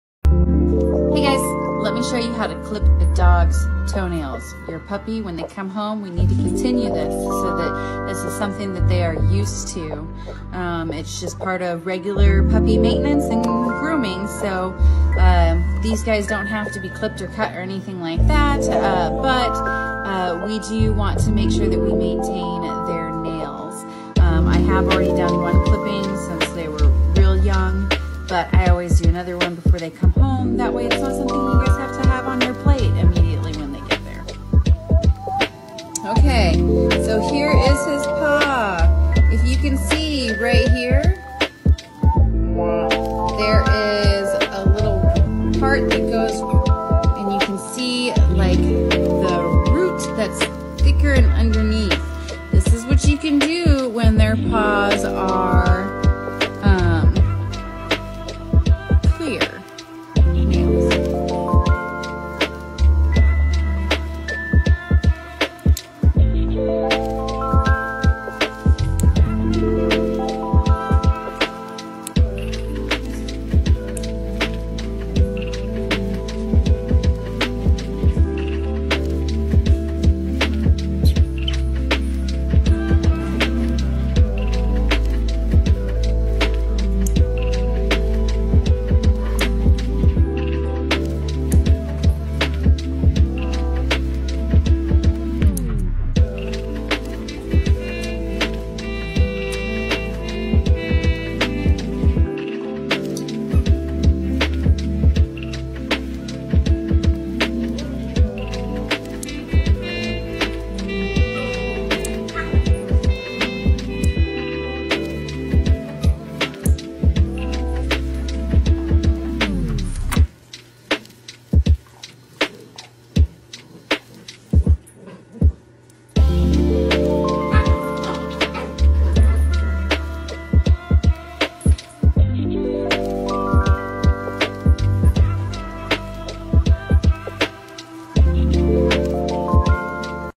Learn how to keep up with your puppies toenails. Everyone is gonna have a fresh cut when they come
Learn how to keep up with your puppies toenails. Everyone is gonna have a fresh cut when they come home, but this will be something that you should continue for them. And it’s best for them to get used to it while they are small.